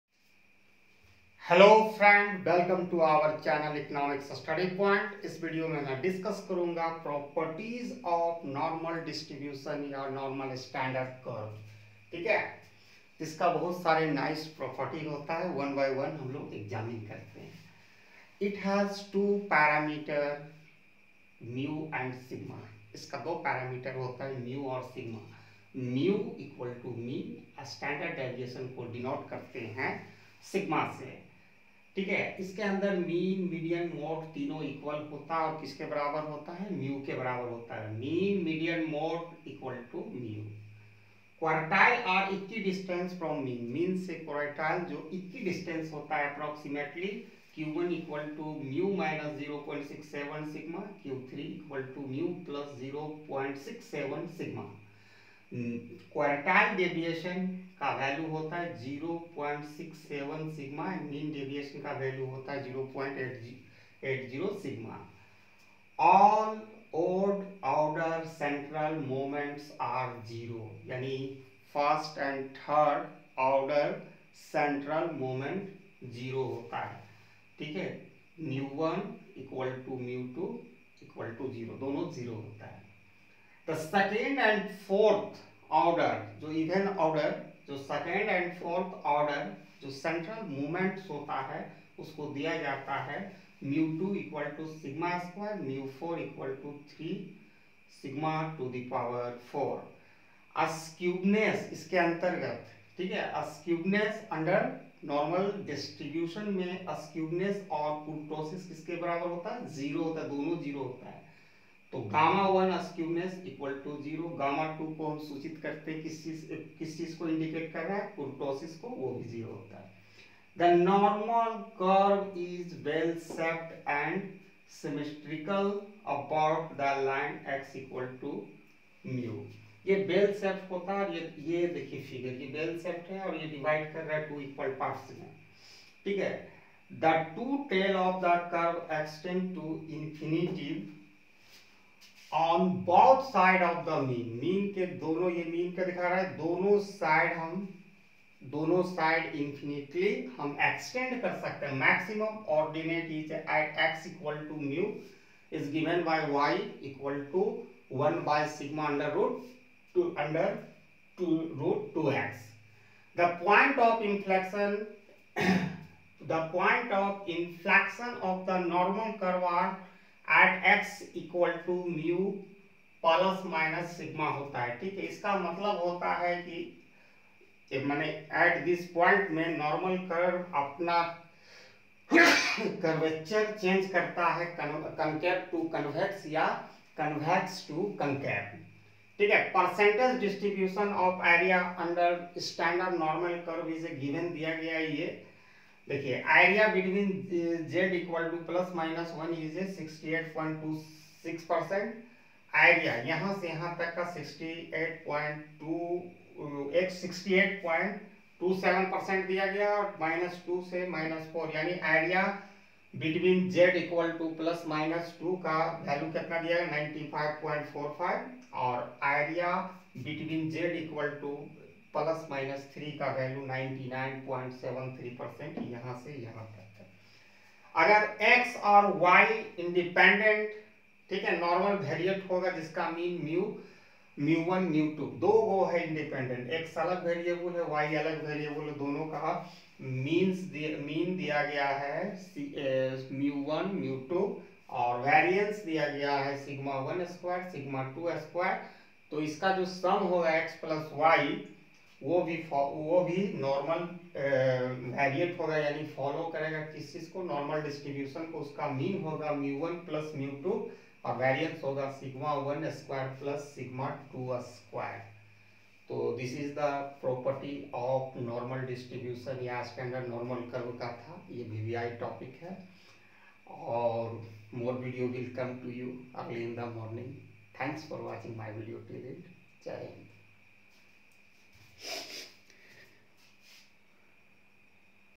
Properties Of Normal Distribution#Standard Normal Curve#MEC-103#IGNOU MA ECONOMICS#.
here to click for Link For Our channel ECONOMICS STUDY POINT

Under this video I have presented a easy comprehensive and complete solution procedure for the question that is frequently asked In Term End Examination IGNOU MA ECONOMICS for course code MEC-003/103 Quantitative Methods

Properties Of Normal Distribution/ Standard Normal Curve

Term End Examination Asked Question Solution

Thanks 😊.